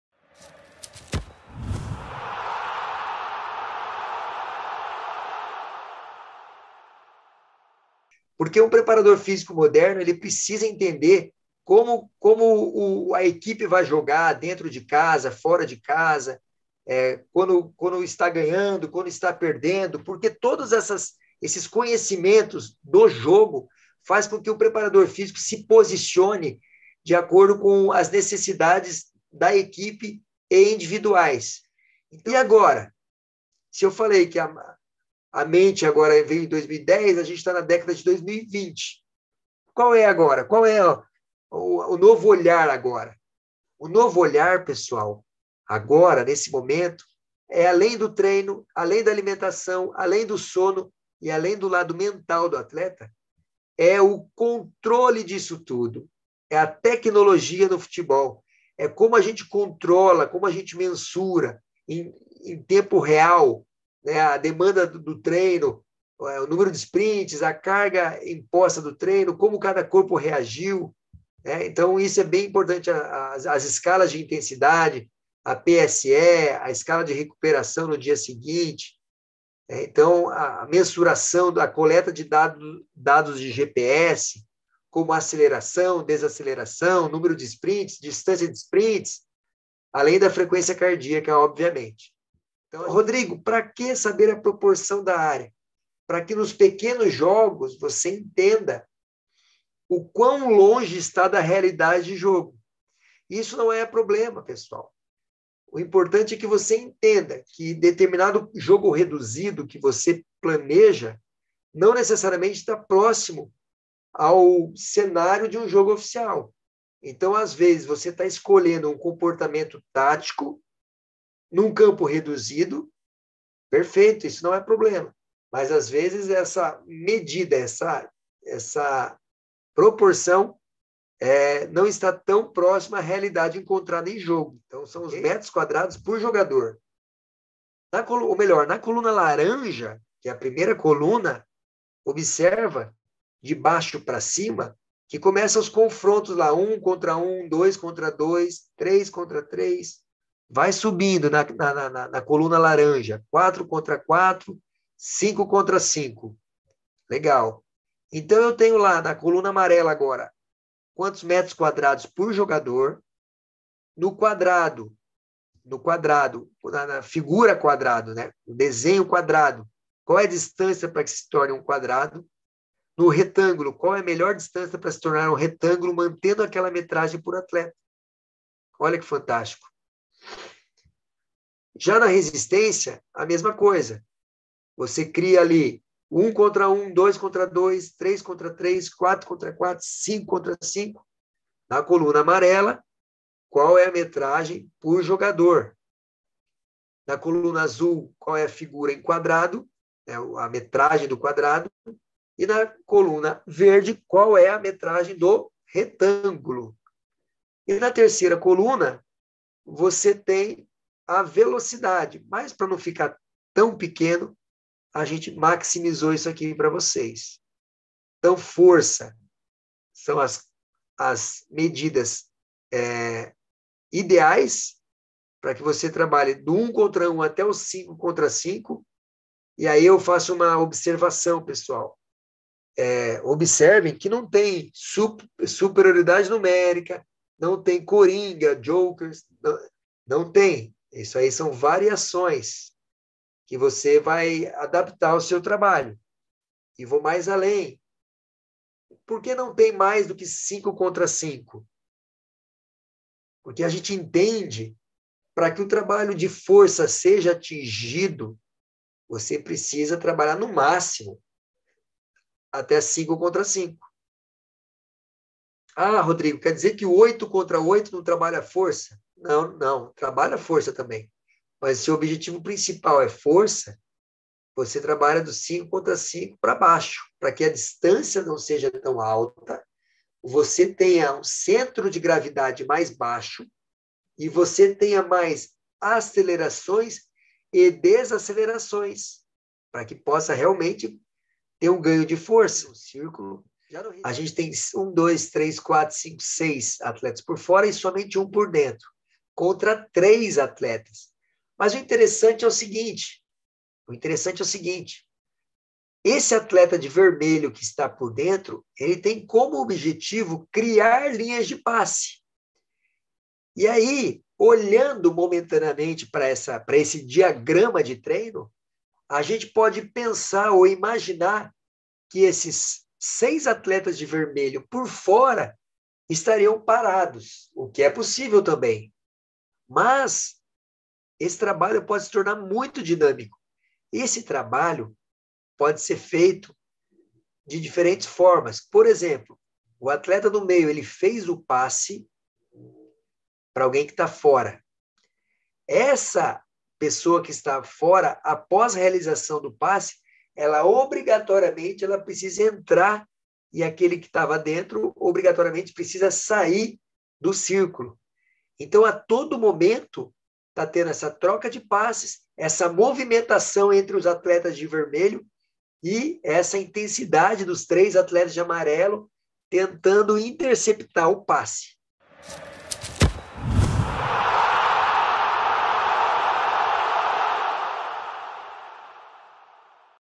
Masterclass Coach ID - Jogos Reduzidos e Preparação Física no Futebol
Breve resumo da Masterclass da Coach ID com o Prof. Rodrigo Rezende, especialista em preparação física com vários anos de experiência em equipas profissionais.